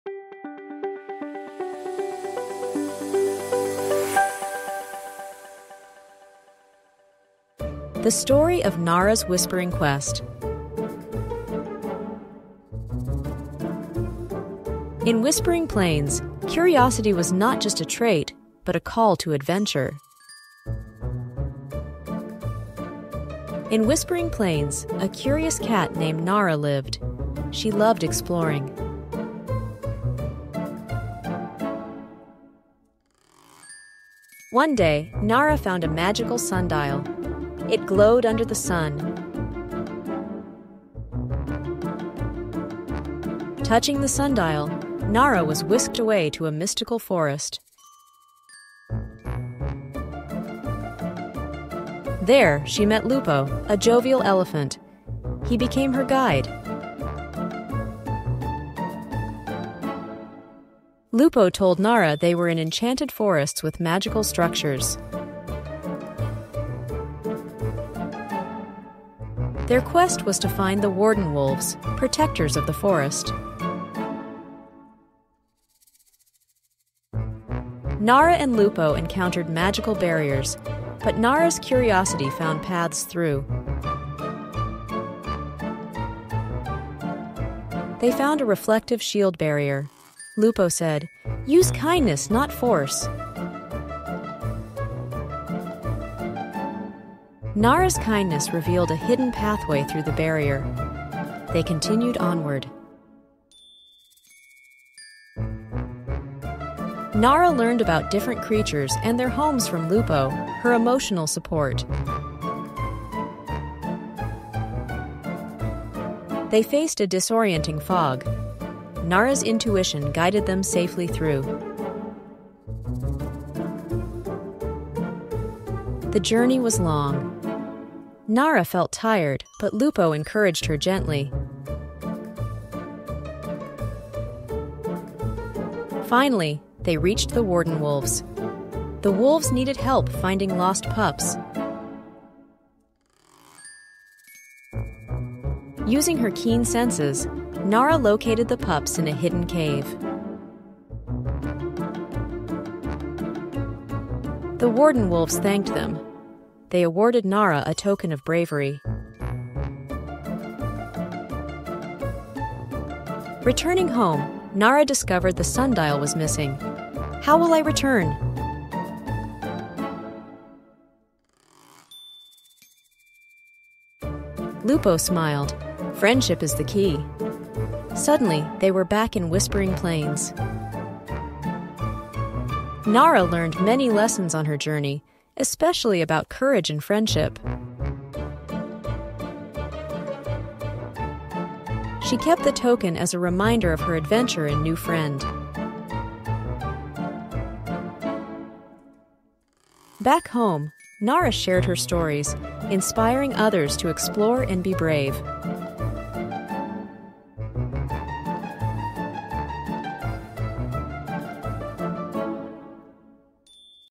Story for Boys and Girls | Nara's Whispering Quest | Bedtime Stories | English
Dive into the enchanting world of "Nara's Whispering Quest," a heartwarming animated story for kids that captures the essence of courage, friendship, and discovery. Join Nara, a curious cat with a spirit of adventure, as she finds herself on an unexpected journey through the magical Whispering Plains. Alongside Lupo, a friendly elephant, they explore mystical lands, face challenges, and learn invaluable lessons that will stay with them forever.

This captivating tale is not just a bedtime story for young children; it's an educational journey through magical tales for kids, offering lessons on bravery, kindness, and the importance of companionship. It's an ideal watch for those seeking adventure tales for children, educational stories for boys and girls, and the joy of story telling.

From the enchanting Enchanted Forest to the wisdom of the Warden Wolves, every moment of Nara's journey is filled with wonder and learning opportunities. Whether it's solving puzzles, overcoming obstacles with kindness, or learning about the magical creatures of this world, "Nara's Whispering Quest" is a treasure trove of hero tales for young children and a testament to the power of curiosity and friendship.

Discover the magic of animated stories for kids, and let Nara's story inspire your little ones to explore, dream, and be brave. Share in her adventures and the lessons learned along the way, making this magical tale a part of your family's story time.